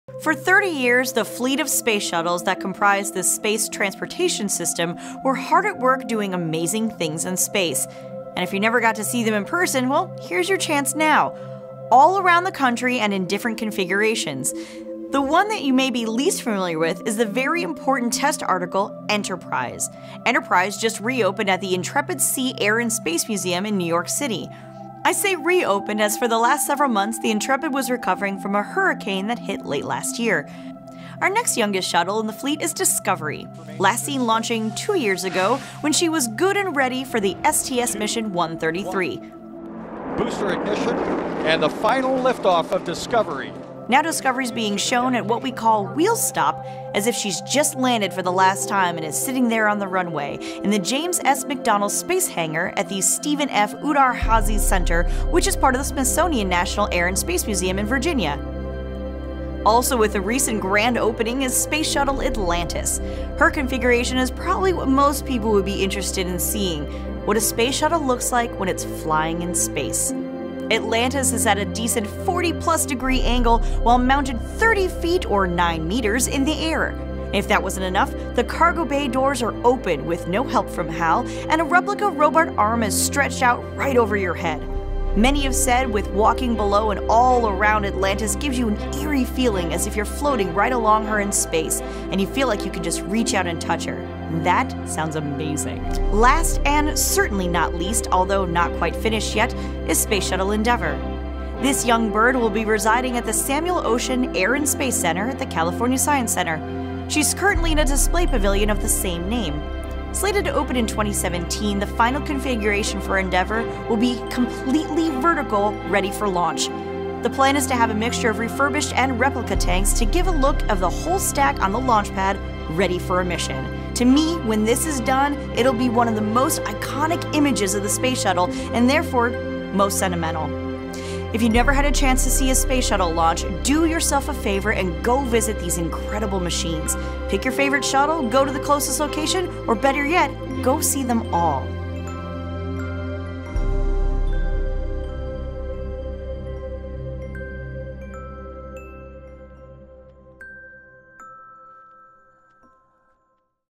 Space Shuttle Exhibits - SpacePod
With the space shuttle fleet now retired, these majestic vehicles have been moved to museums. We'll take a quick look at the remaining 4 (not 3) orbiters, the layout they are now in and where you can see them.